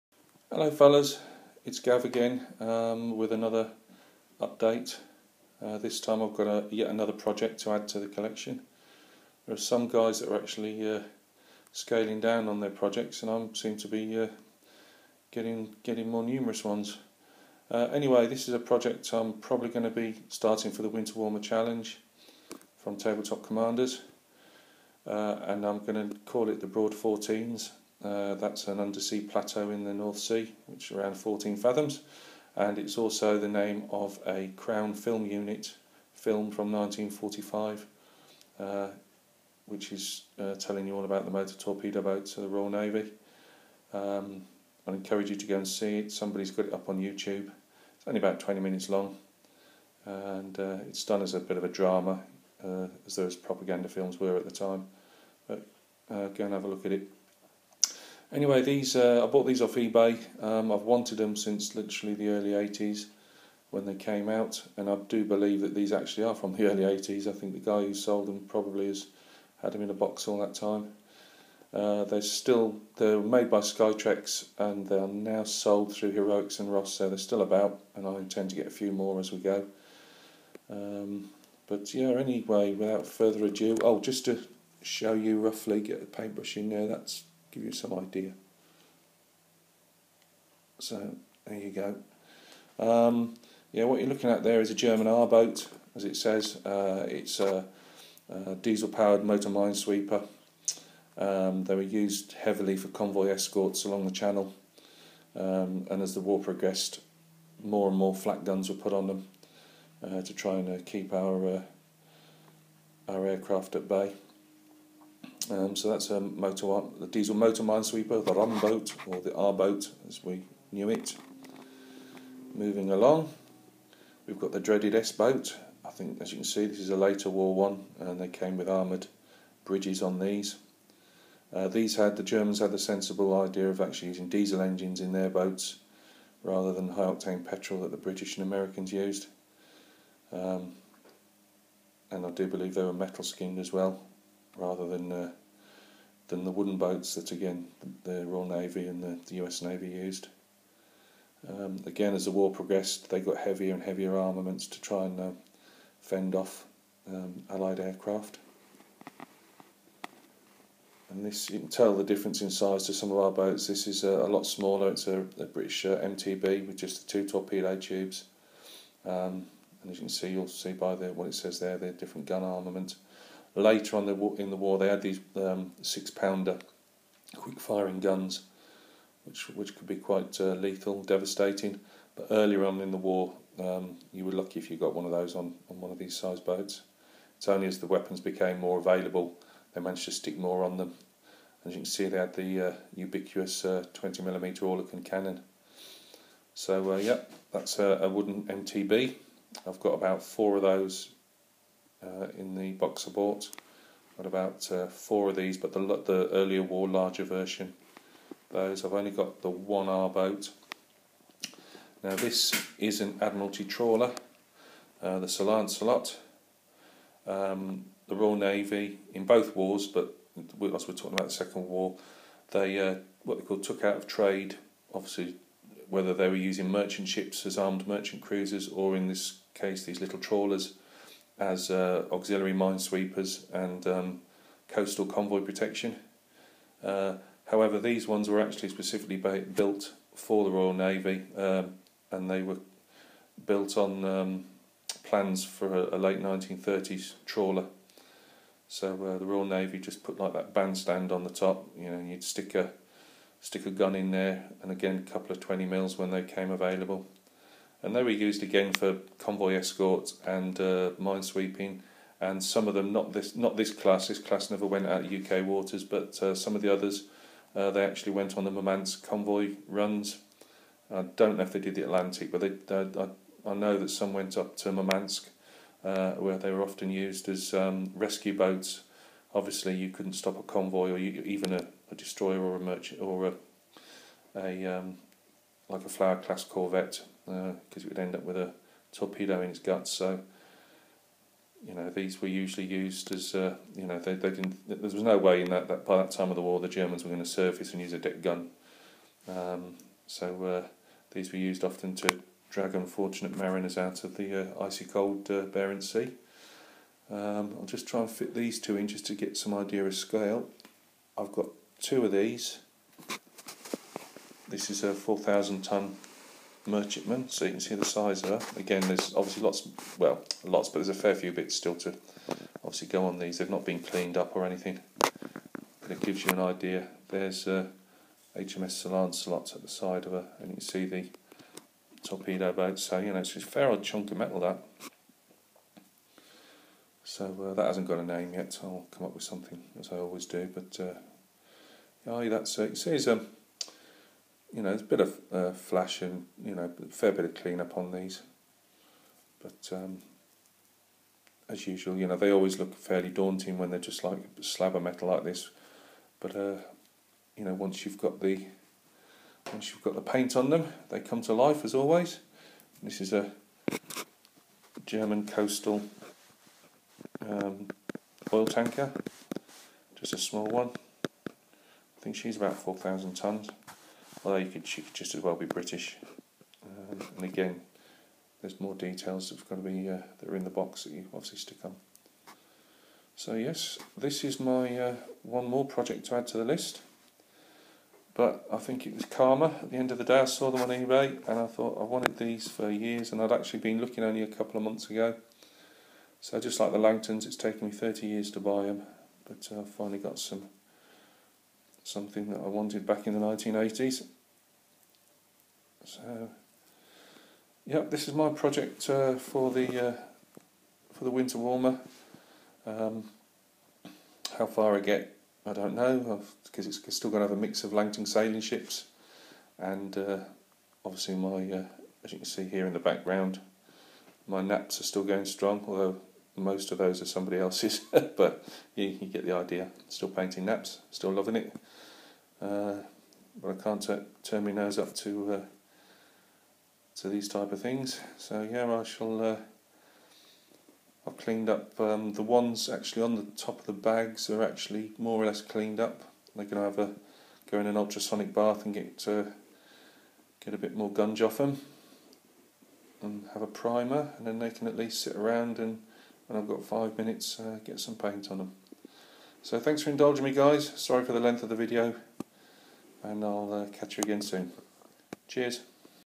The Broad Fourteens (my winter warmer challenge )
Hi all these are my 1/600 Light Coastal forces there's not much to see yet but I thought I would give you a look at another part of history I enjoy . Thanks for stopping by . Regards Gav .